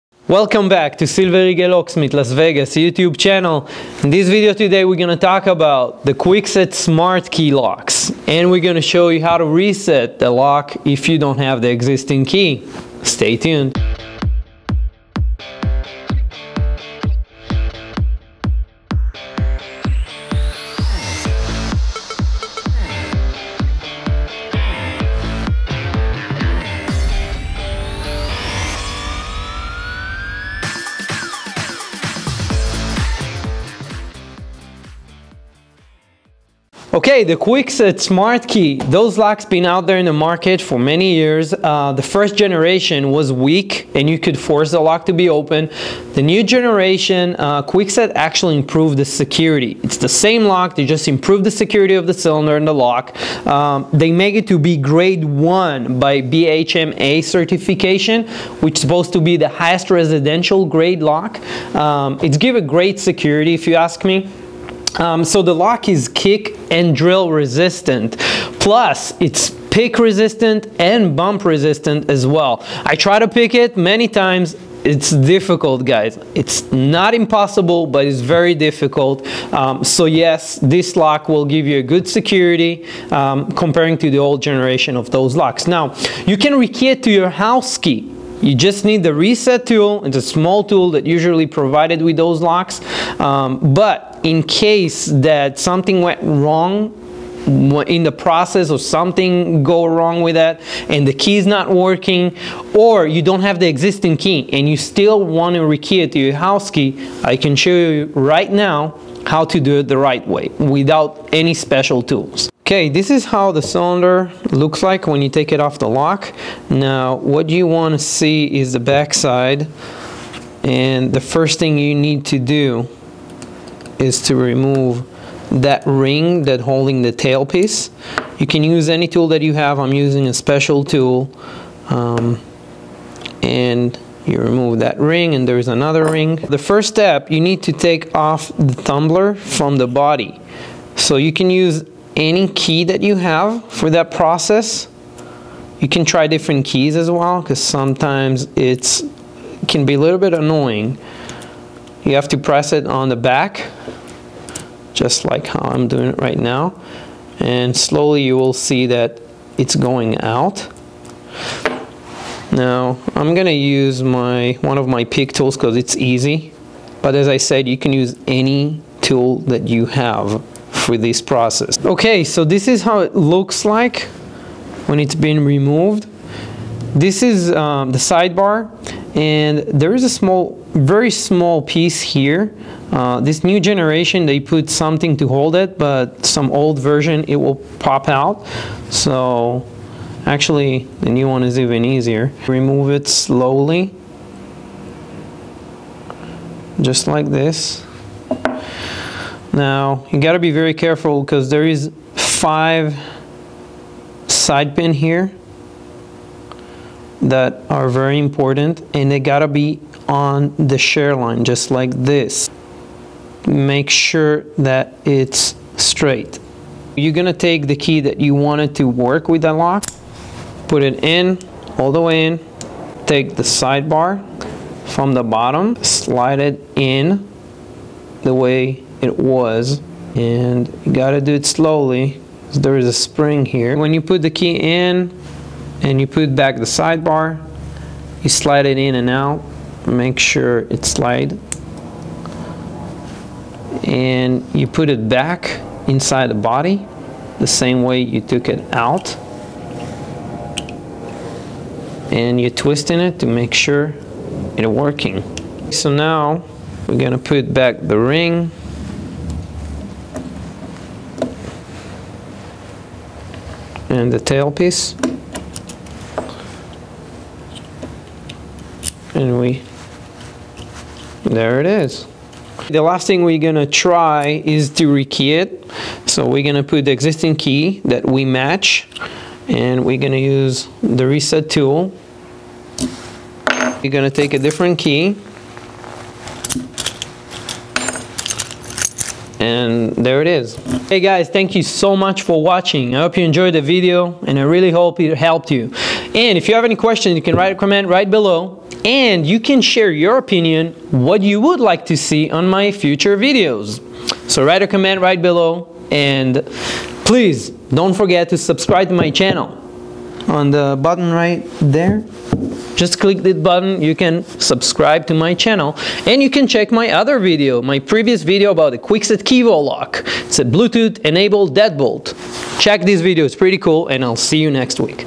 How To Reset Kwikset Smart Key Without the Current Key Or The Reset Tool ✅
Learn how to reset Kwikset Smart Key when you don't have the current key or the reset tool & rekey it to your house key.
🡻 Click SHOW MORE 🡻 for all the Links 🡻

Recovering a Misprogrammed SmartKey Cylinder

kwikset rekey
kwikset smart key reset
smartkey
kwikset smartkey
5 Tools You Require When Resetting Your Kwikset Smart Key Lock.

One of the best things about a Kwikset Smart Key Lock is the fact that you can very easily reset the key. This means that you have the chance to take any old useless key in the house and turn it into the main key. This comes in handy when you already have the copies of the old key as it is more convenient for everyone than having to make new copies. It is also useful when you want to render someone’s key useless without having to change the entire lock. Either way, the feature is something very useful. Lock resetting is a pretty easy project and is something that you can try if you don’t mind getting your hands a little dirty. But before you get into the entire process, you will need to have your tools. Below is all that you need for successful key resetting and why you need it.

1. Screw driver

The first thing that you will need when resetting your key is the appropriate screw driver. The term appropriate here means that the screw driver matches the screw heads on the lock. The screw driver will help you take apart the lock in order to gain access to the lock cylinder to make the necessary changes.

2. The existing lock cylinder

This is the most important part in this entire project. The existing lock cylinder contains the parts that make it possible to rest the key. This includes the tumbler barrel. You will get the lock cylinder by using the screw driver to take apart the lock. You don’t have to take the entire lock apart. You just need to release the part with the cylinders.

3. The keys

You will need to sets of keys. The first key is the specific key designed by the manufacturer for your lock. This comes in handy in the first few steps aimed at reconfiguring the tumblers. The second key is the old key that you intend to use as the new key for your lock.

4. Tail piece removers

The process of lock resetting involves taking the cylinder apart and you will need appropriate tools to do this. The tail piece remover allows you to safely remove the tail piece and the rings from the cylinder and to accurately replace them once the job is done.

5. A metallic pick

This is used to remove the tumbler from the cylinder. The metallic pick is usually a part of a locksmith’s set of picking tools. Unless you are licensed, handling one of these could get you in a lot of trouble. You can therefore substitute it using the existing key. It will be a little more difficult but is a better option than jail.

6. Reset tool

Finally, you will need the resetting tool. This is what you use to configure the tumblers to the specifications of the key you want to use.

Bottom line

The best part about these tools is the fact that they are all very easily available and even more easy to use. This ensures that you do it yourself resetting process runs as smoothly as possible. All you need is to ensure that you know all the steps and that you know which instrument to use in the different steps. If you have all this information, the process should be fairly simple and massively successful.